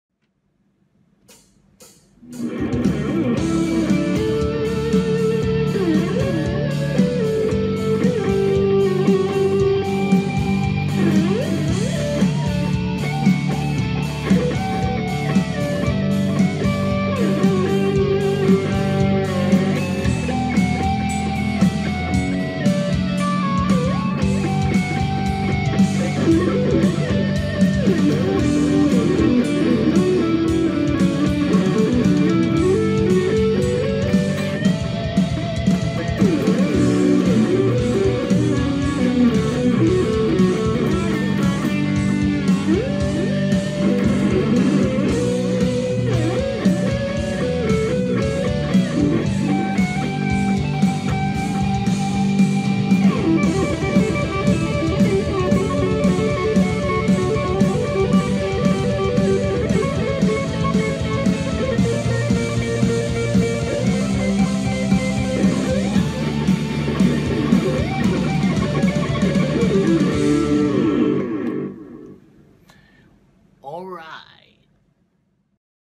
TugtuganSiFDJ
Thanks master @juwanfidle09 for the chance to jam on your backing track. I'm not currently in PH so this basically is not qualified as an entry to the contest (lol). Keep up the passion, FDJ! Rock on. God bless.